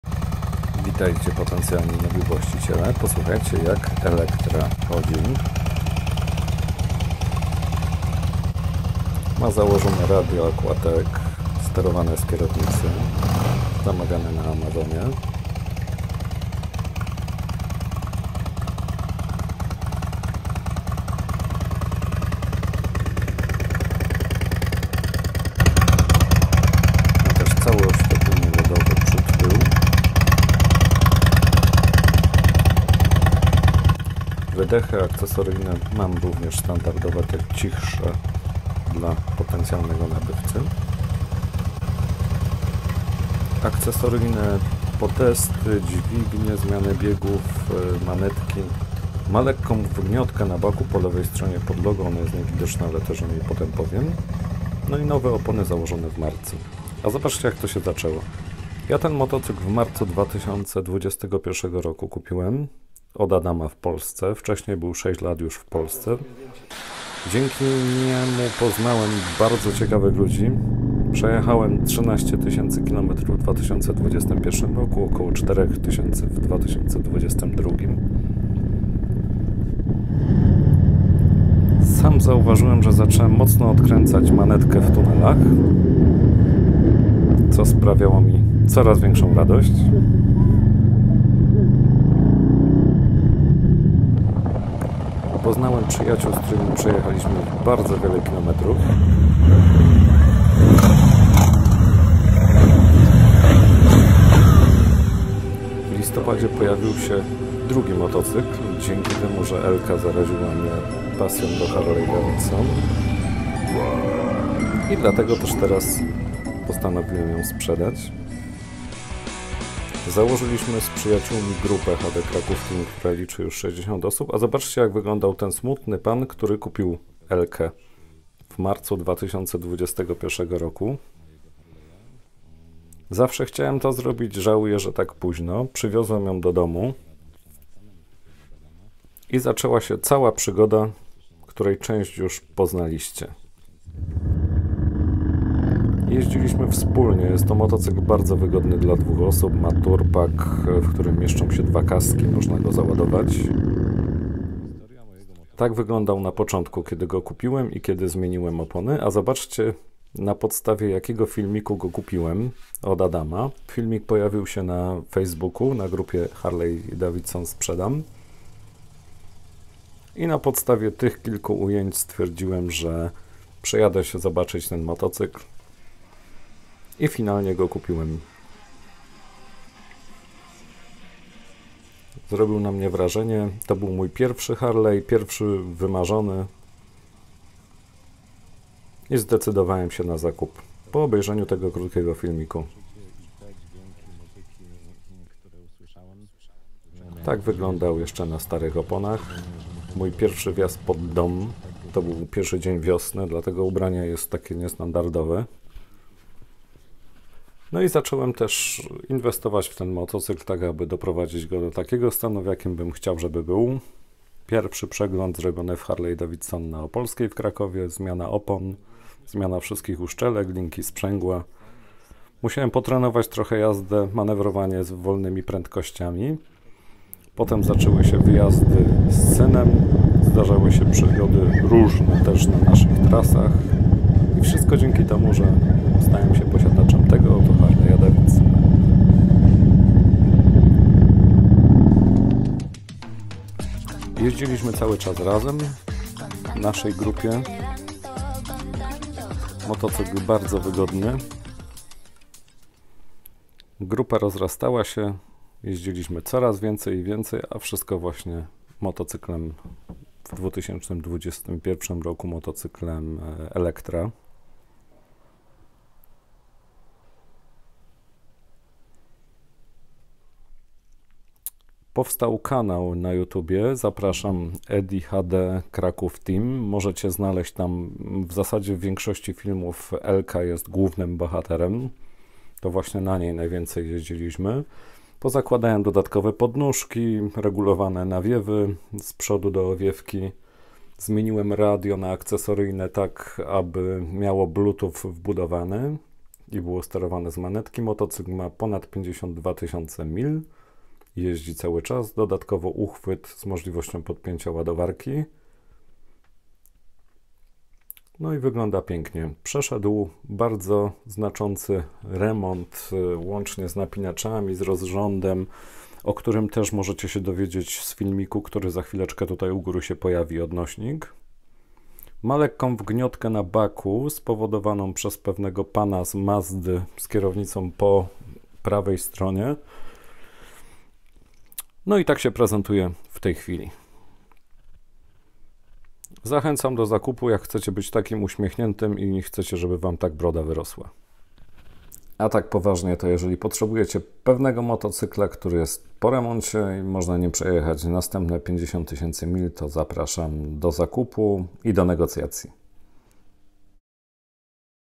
Sprzedam Elkę - Harley Electra 2006
Aktualizacja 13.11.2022 - Elka trafiła już do nowego właściciela - cieszę się że będzie w dobrych rękach 👍👍

Sprzedam mój ukochany motocykl Harley-Electra 2006. Podjąłem taka decyzję z uwagi na drugi motocykl touring który kupiłem.
Zapraszam do zakupu, motocykl jest głównym bohaterem wielu moich filmów na kanale Edi HD Kraków Team.
Szczegóły:
Electra 2006, 88 cali, 1450 cm3 pojemności.
Jestem jej właścicielem od 20 marca 2021 roku. Motocykl był wcześniej od 6 lat w Polsce a pochodzi z USA. Ubezpieczenie AC/OC do marca 2023, przegląd do lipca 2023.
W październiku 2022 wykonany został gruntowny przegląd/remont -
poniżej link do filmu zawierającego szczegóły

Zapraszam do zakupu - mój nr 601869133 a gdybym nie odbierał to oddzwonię po pracy - cena 44 tys zł do negocjacji
Edi